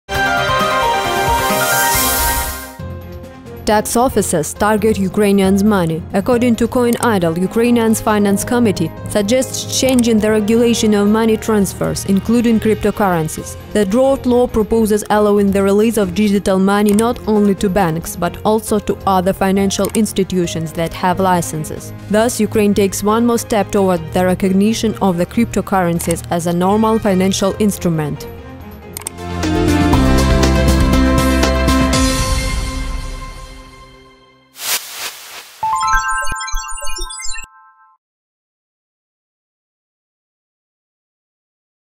KCN Ukrainians may start paying personal income tax on cryptocurrency
Steemit.com/@kcnnews

Tax officers target Ukrainians' money. According to CoinIdol, Ukraine's Finance Committee suggests changing the regulation of money transfers, including cryptocurrencies. The draft law proposes allowing the release of digital money not only to banks, but also to other financial institutions that have licenses. Thus, Ukraine takes one more step towards the recognition of the cryptocurrencies as a normal financial instrument.